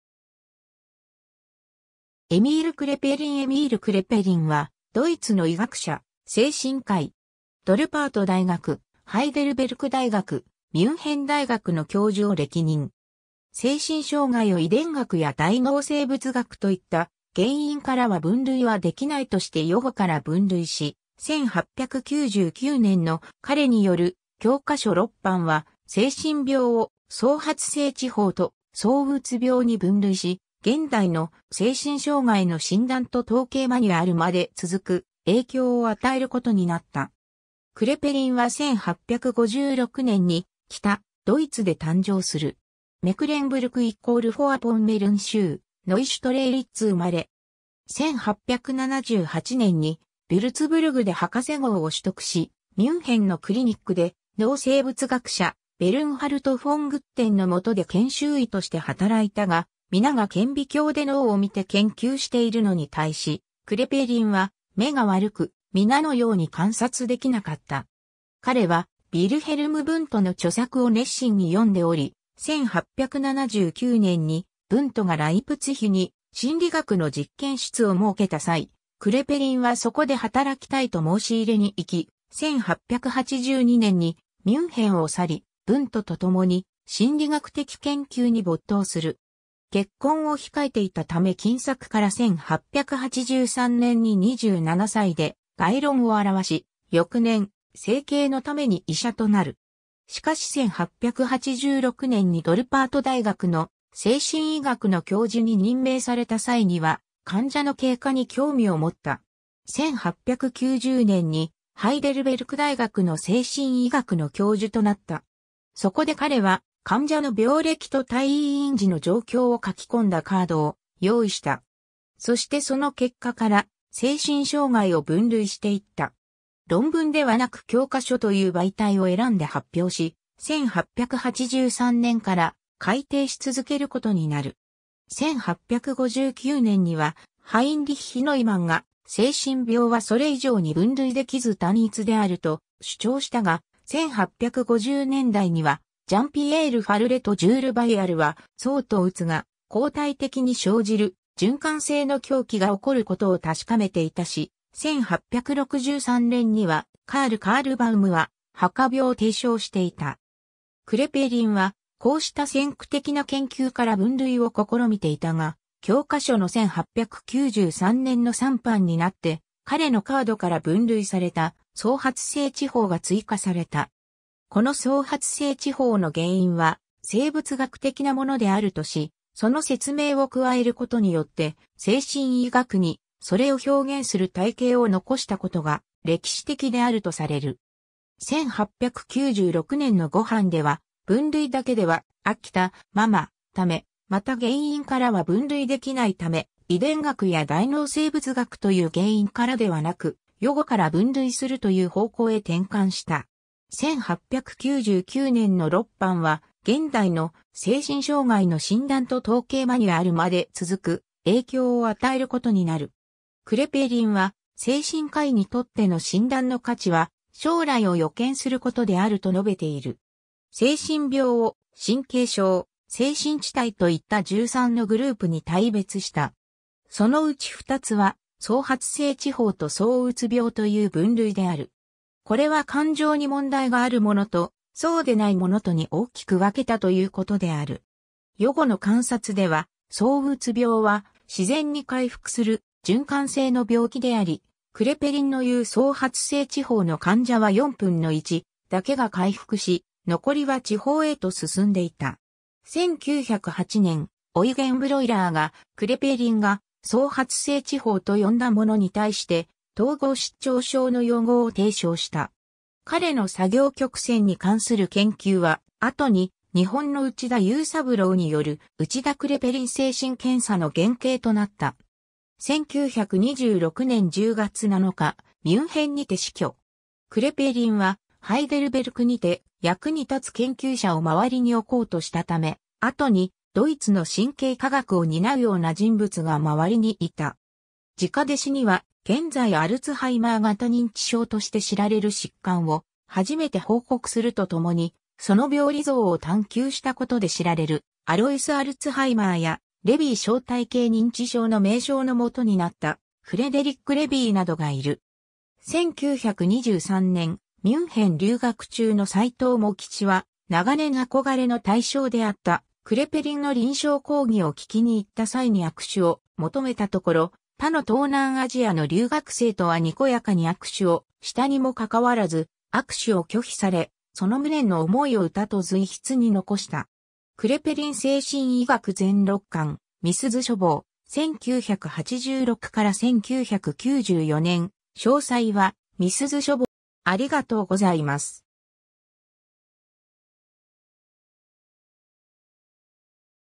エミール・クレペリン
ルートヴィヒ・マクシミリアン大学ミュンヘンの教員
ルプレヒト・カール大学ハイデルベルクの教員
エミール・クレペリン エミール・クレペリン（Emil Kraepelin, 1856年2月15日 - 1926年10月7日）は、ドイツの医学者、精神科医。
ドルパート大学（現・エストニア国立タルトゥ大学）、ハイデルベルク大学、ミュンヘン大学の教授を歴任。
 精神障害を遺伝学や大脳生物学といった原因からは分類はできないとして予後から分類し、1899年の彼による教科書6版は、精神病を「早発性痴呆」（現在の統合失調症、旧称「精神分裂病」）と「躁うつ病」（現在の双極性障害）に分類し、現代の『精神障害の診断と統計マニュアル』(DSM)まで続く影響を与えることになった。
 1856年に北ドイツで誕生する。
メクレンブルク＝フォアポンメルン州ノイシュトレーリッツ生まれ。
 1878年にヴュルツブルクで博士号を取得し、ミュンヘンのクリニックで脳生物学者ベルンハルト・フォン・グッデン（de:Bernhard von Gudden）のもとで研修医として働いたが、皆が顕微鏡で脳を見て研究しているのに対し、クレペリンは目が悪く、皆のように観察できなかった。
 彼はヴィルヘルム・ヴントの著作を熱心に読んでおり、1879年にヴントがライプツィヒに心理学の実験室を設けた際、クレペリンはそこで働きたいと申し入れに行き、1882年にミュンヘンを去りヴントと共に心理学的研究に没頭する。
結婚を控えていたため金策から1883年に27歳で『概論』を著し、翌年、生計のために医者となる。
しかし1886年にドルパート大学の精神医学の教授に任命された際には、患者の経過に興味を持った。
 1890年にハイデルベルク大学の精神医学の教授となった。
そこで彼は患者の病歴と退院時の状況を書き込んだカードを用意した。
そしてその結果から精神障害を分類していった。
論文ではなく教科書という媒体を選んで発表し、1883年から改訂し続けることになる。
 1859年には、ハインリッヒ・ノイマン（de:Heinrich Neumann (Psychiater)）が、精神病はそれ以上に分類できず単一であると主張したが、1850年代にはジャン=ピエール・ファルレ（fr:Jean-Pierre Falret）とジュール・バイヤルは躁とうつが交代的に生じる「循環性の狂気」が起こることを確かめていたし、1863年にはカール・カールバウム（de:Karl Ludwig Kahlbaum）は破瓜病を提唱していた。
クレペリンはこうした先駆的な研究から分類を試みていたが、教科書の1893年の3版になって、彼のカードから分類された早発性痴呆（現在の統合失調症）が追加された。
この早発性痴呆の原因は生物学的なものであるとし、その説明を加えることによって精神医学にそれを表現する体系を残したことが歴史的であるとされる。
1896年の5版では、分類だけでは「飽きた〔ママ〕」ため、また原因からは分類できないため、遺伝学や大脳生物学という原因からではなく、予後から分類するという方向へ転換した。
 1899年の6版は、現代の『精神障害の診断と統計マニュアル』(DSM)まで続く影響を与えることになる。
クレペリンは精神科医にとっての診断の価値は、将来を予見することであると述べている。
精神病を神経症、精神遅滞といった13のグループに大別した。
そのうち2つは、早発性痴呆と躁うつ病（現在の双極性障害）という分類である。
これは感情に問題があるものと、そうでないものとに大きく分けたということである。
 予後の観察では、躁うつ病は自然に回復する循環性の病気であり、クレペリンのいう早発性痴呆の患者は4分の1だけが回復し、残りは痴呆へと進んでいた。
1908年、オイゲン・ブロイラーが、クレペリンが「早発性痴呆」と呼んだものに対して「統合失調症」の用語を提唱した。
 彼の作業曲線に関する研究は、後に日本の内田勇三郎による内田クレペリン精神検査（クレペリン検査）の原型となった。
 1926年10月7日、ミュンヘンにて死去。
 クレペリンはハイデルベルクにて役に立つ研究者を周りに置こうとしたため、後にドイツの神経科学を担うような人物が周りにいた。
 直弟子には、現在アルツハイマー型認知症として知られる疾患を初めて報告するとともに、その病理像を探求したことで知られるアロイス・...